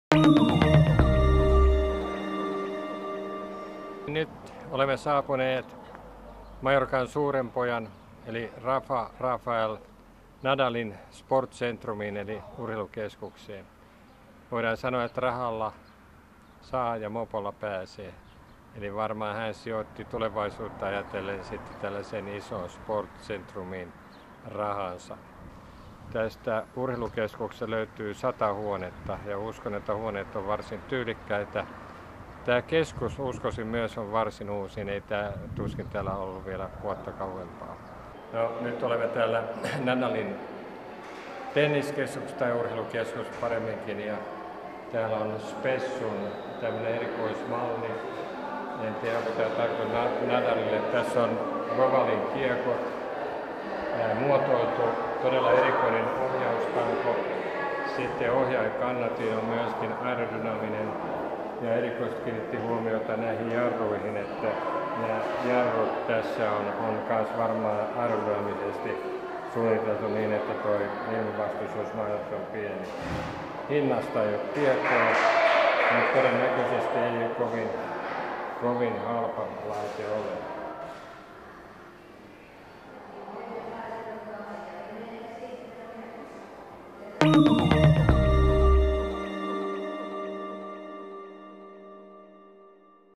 Alcudia, Mallorca. Pyöräleiri, kevät 2017. Rafa Nadal Sport Center.
Pyöräleirin ohesssa kävimme Kari Mattilan kanssa Rafa Nadalin Sport Centerissä Manacorissa Mallorcalla.